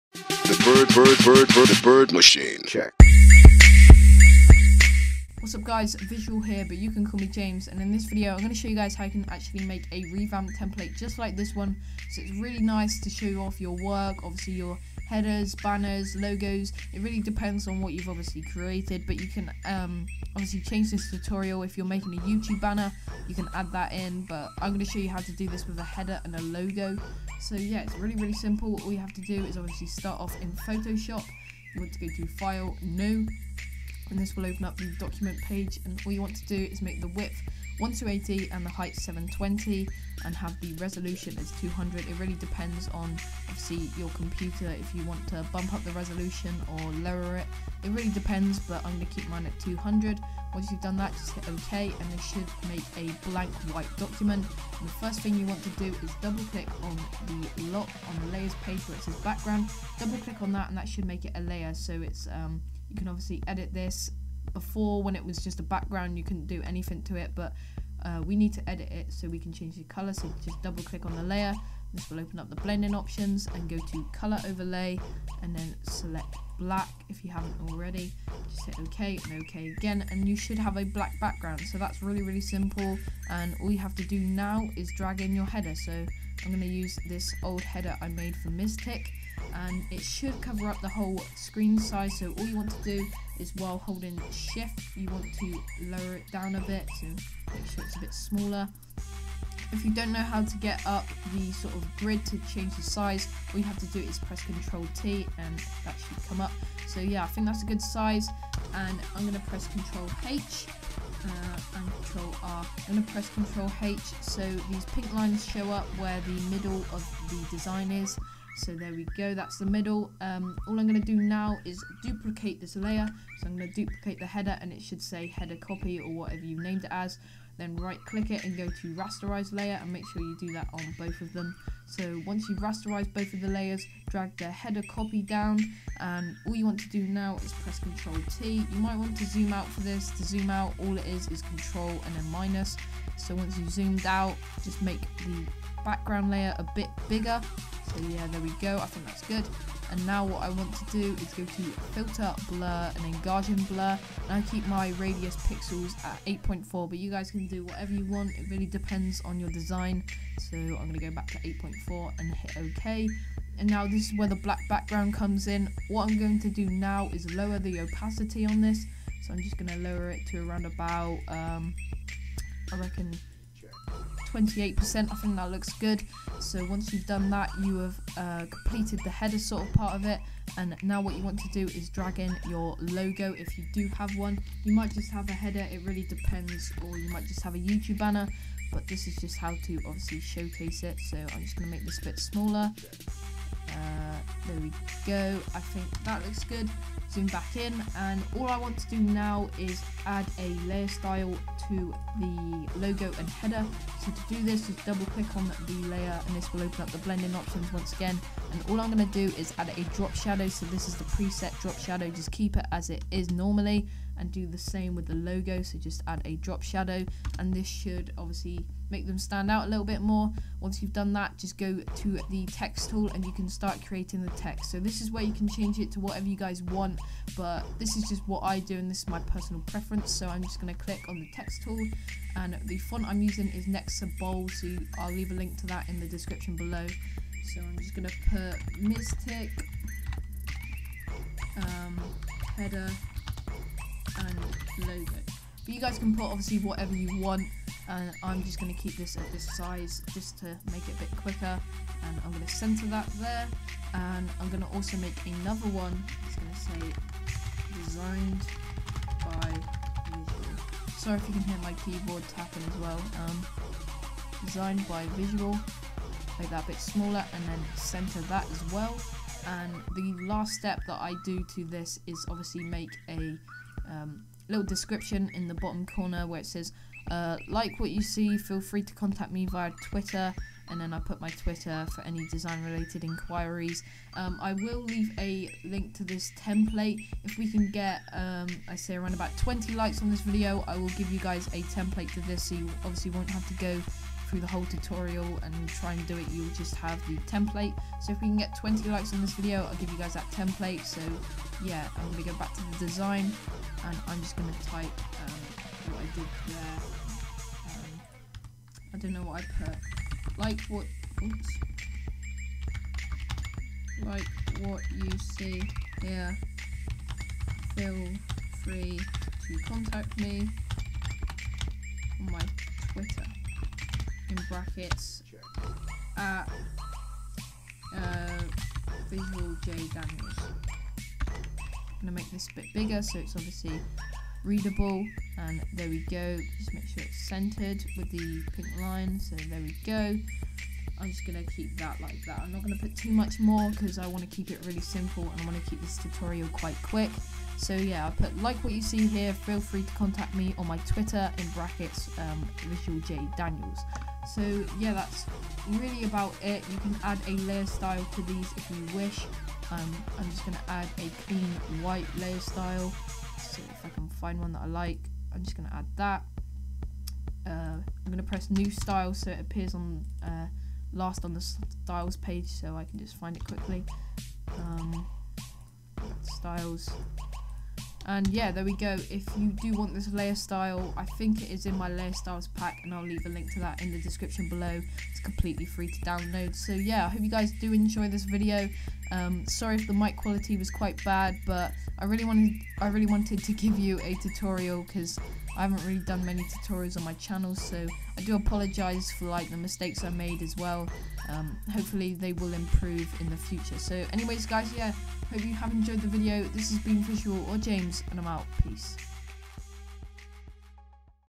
12 Days of Christmas #8: How to Make a Rebrand/Revamp Layout (Basic)
Welcome to Day 8 of '12 Days of Christmas', where I show you 'How to make a Rebrand/Revamp Layout'...

Font Used: Nexa Bold

Thanks for 20 Likes!